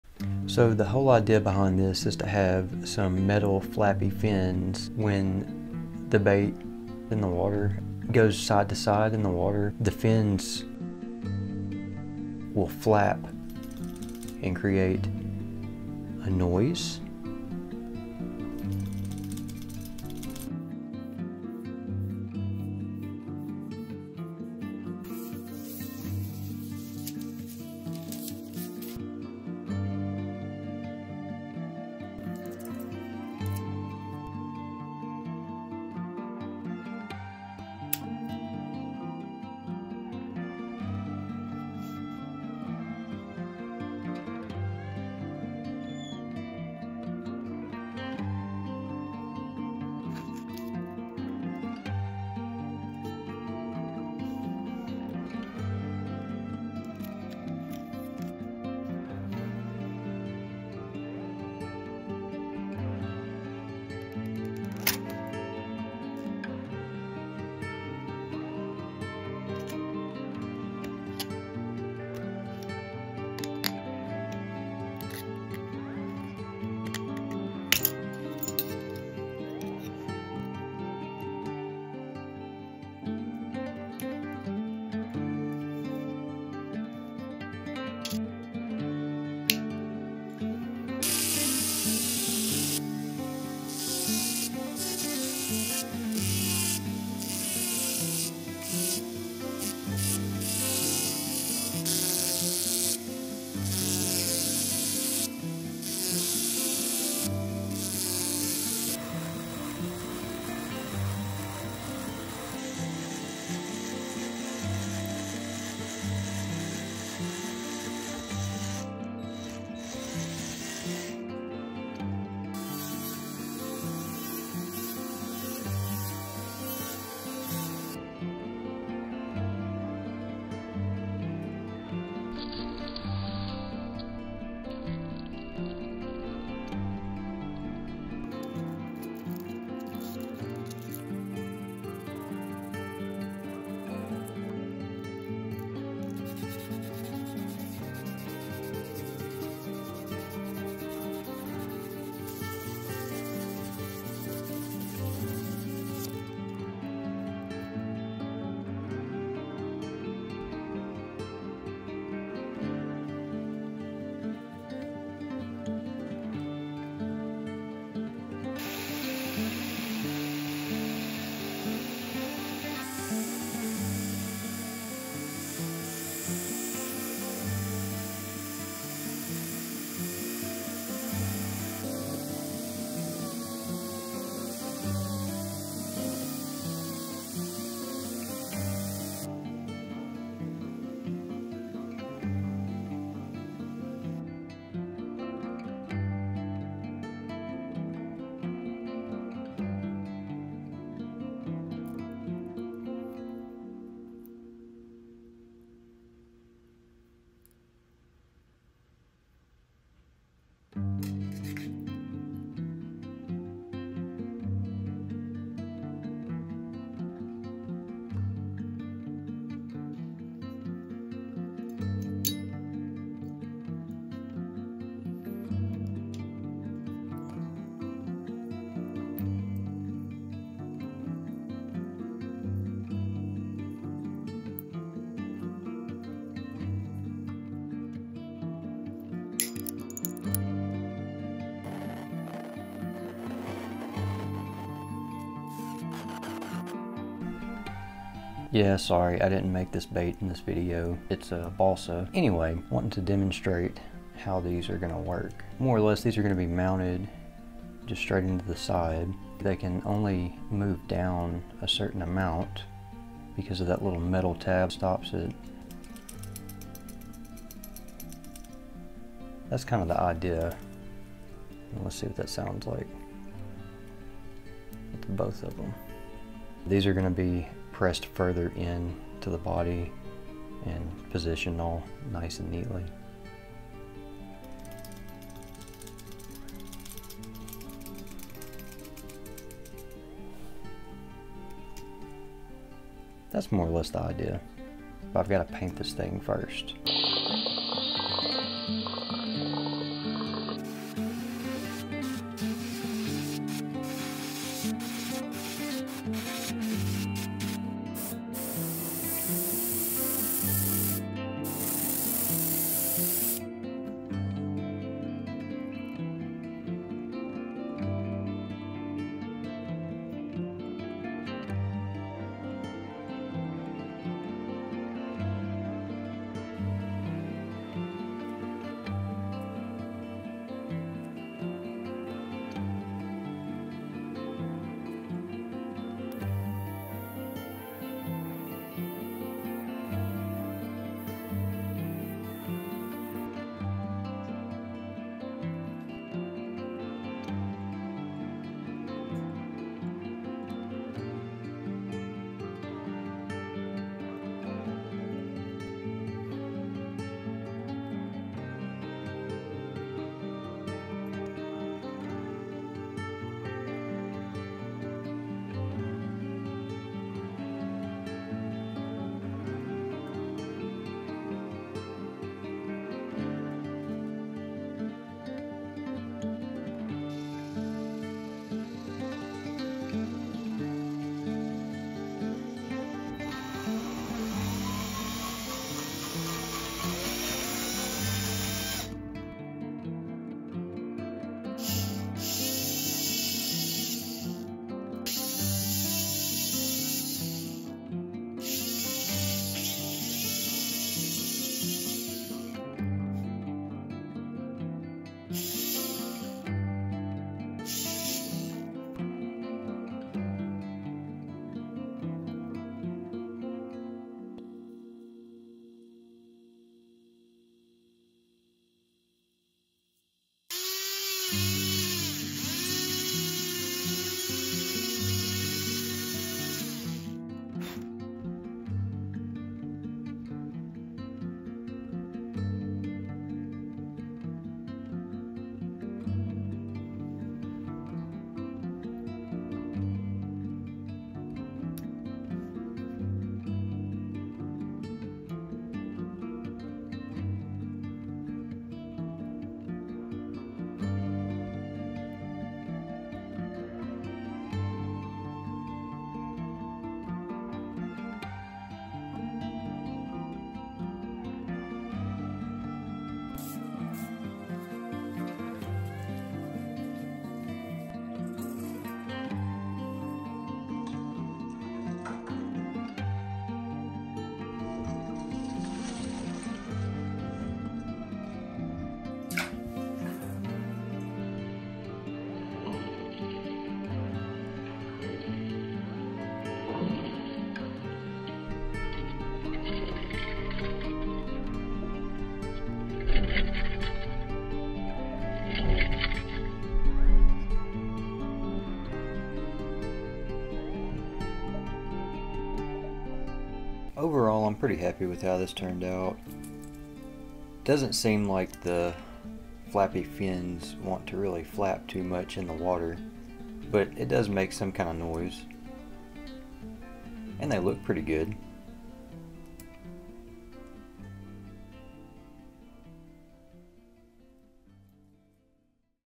Making Metal Fishing Lure Fins
For this lure making video I wanted to do something innovative and try to add some metal flappy fins to the side of a Crankbait.

Overall I’m happy with how it turned out but not so happy with the amount of flap I got out of them.

Below is a list of some of the tools and materials I use.

Water Slide Decal Paper
Utility Knife
Disc Sander
Drill Press
Lip Material
Stainless Wire
Poplar Wood
Airbrush Paint
Clear Coat
Rotary Tool